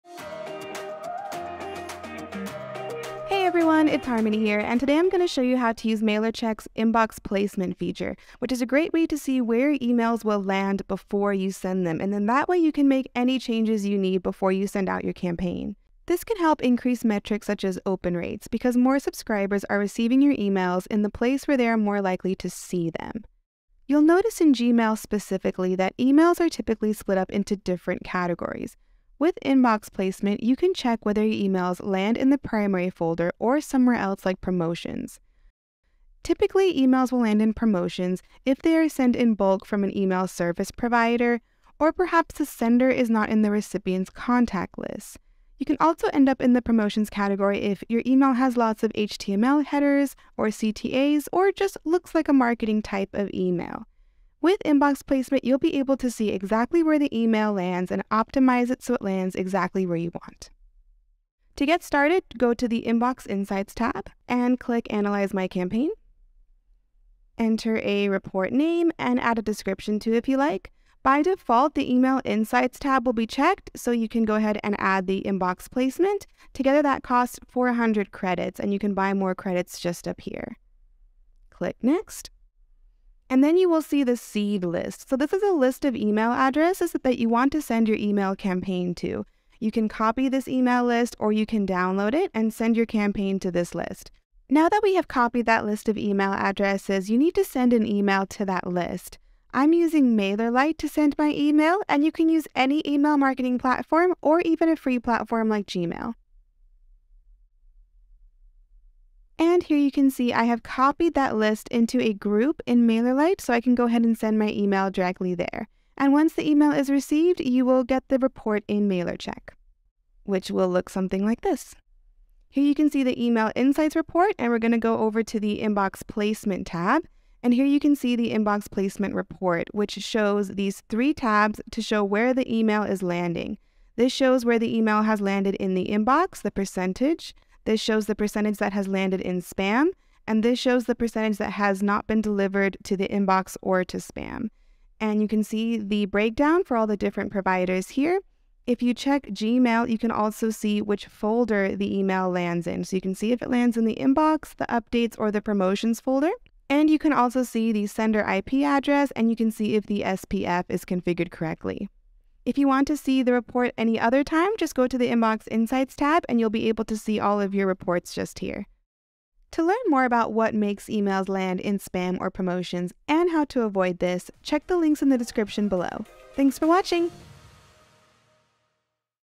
Inbox placement: How to test where email lands
In this video, we address a common frustration many marketers face: email placement issues. If you've ever struggled with your emails ending up in spam or the promotions tab, we have the perfect solution for you. Introducing MailerCheck's inbox placement feature, a game-changer in the world of email marketing.

⭐️ Sign up with MailerCheck using this link and get 200 free credits

Join us as we demonstrate how MailerCheck can help you overcome email placement challenges. With this powerful tool, you can test the placement of your emails before sending them out, ensuring maximum deliverability and visibility. By analyzing your campaign, MailerCheck provides valuable insights into whether your emails land in the primary folder or get buried in promotions.

Optimizing your inbox placement can have a significant impact on your open rates and overall engagement. Imagine the increased effectiveness of your email marketing campaigns when your messages are seen by the right audience at the right time.

Time stamps:
0:00 - Benefits of using the Inbox placement feature to test where your email lands
0:35 - Why some emails can land in Gmail's Promotions folder
1:01 - Start the Inbox placement test
1:22 - What to do with the seed list provided
1:34 - Sending your email campaign to the seed list emails
2:00 - Open the Inbox placement report in MailerCheck